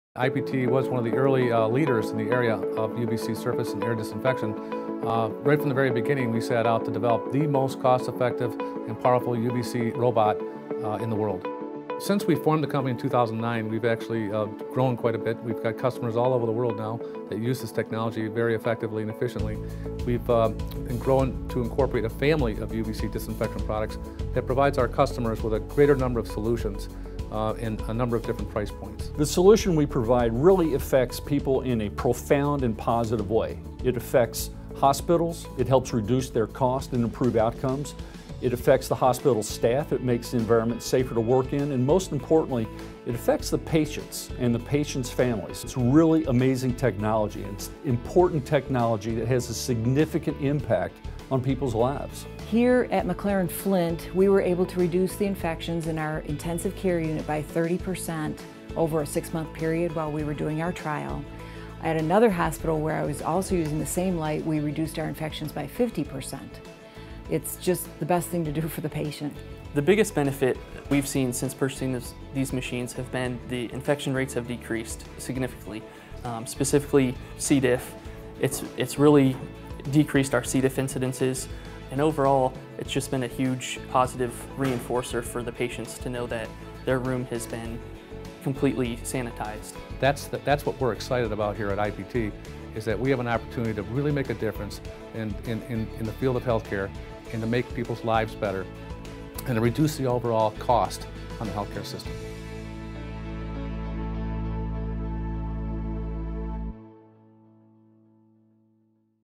Infection Prevention Technologies: Saving lives with UVC Disinfection
Driven to eradicate hospitals of life-threatening healthcare acquired infections (HAIs), IPT has developed a family of UVC disinfection robots, including the most cost-effective and powerful germicidal UV robot in the world - the IPT 3200 Now operating in hospitals and other healthcare environments globally, the company's Next Gen UVC disinfection robots are validated by science, with UV light disinfection trials at various U.S. hospitals and a far-reaching hospital-wide study that showed that IPT's UV light disinfection technology can reduce HAIs by as much as 34%.  Learn more about how IPT UV disinfection robots help hospitals reduce healthcare costs and improve patient outcomes